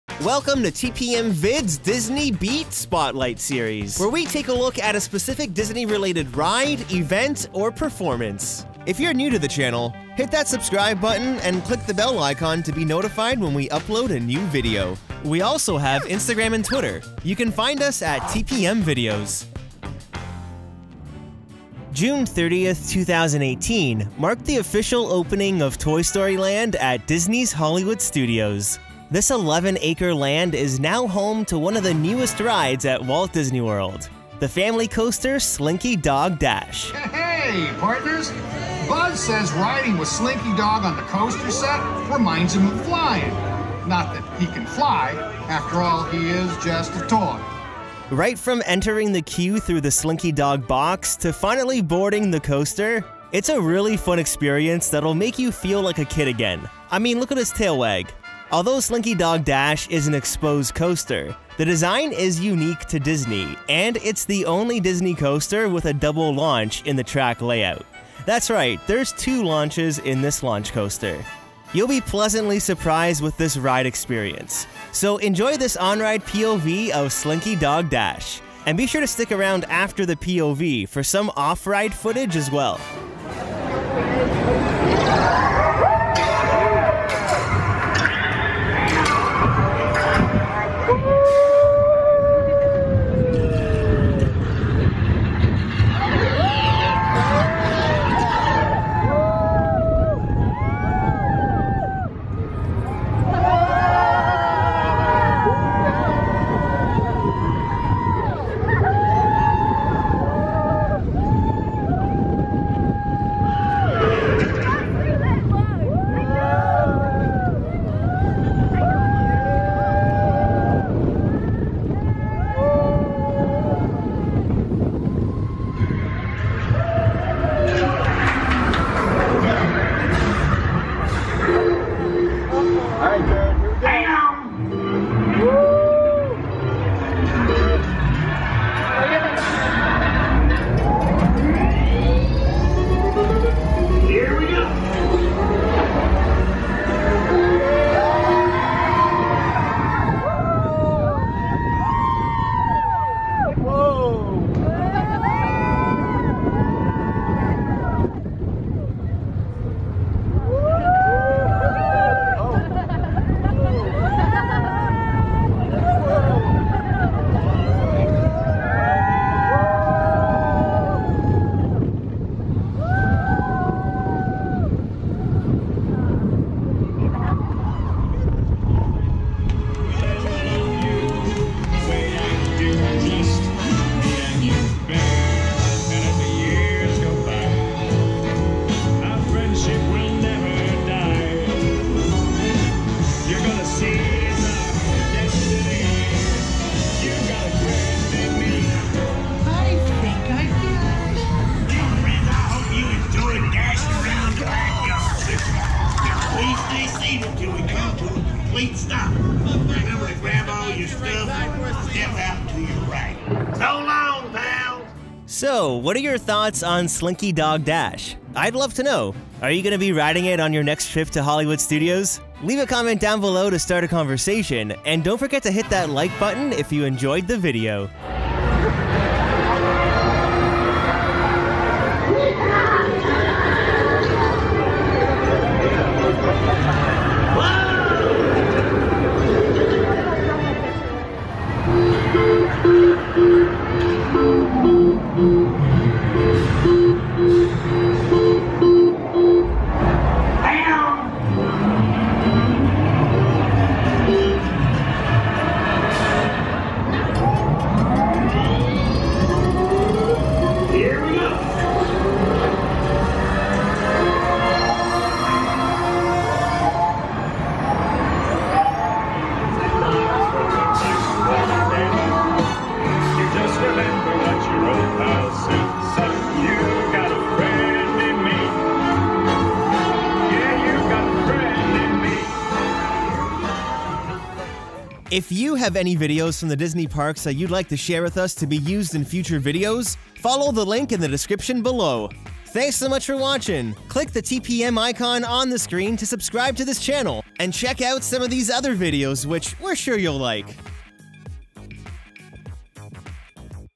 Slinky Dog Dash FULL Front Seat POV On Ride- Toy Story Land Disney World
Slinky Dog Coaster- Get ready for one of the best front seat POVs of Slinky Dog Dash! All your favorite Toy Story characters come to life in Toy Story Land at Disney's Hollywood Studios! One of the new rides of this 11 acre land is the roller coaster Slinky Dog Dash. This Disney ride is perfect for the entire family! Slinking Dog Dash is a launch coaster that has not one, but two launches making it one of the fastest rides at Walt Disney World. It's a really fun ride with some moments of airtime and you get some great views of the the theme park. The ride also includes an animatronic of Wheezy from the Toy Story films! So sit back, keeping your hands, arms feet and legs inside the train at all times and get ready for a front seat Slinky Dog coaster POV!

*Have a video you want to share? Submit it here!*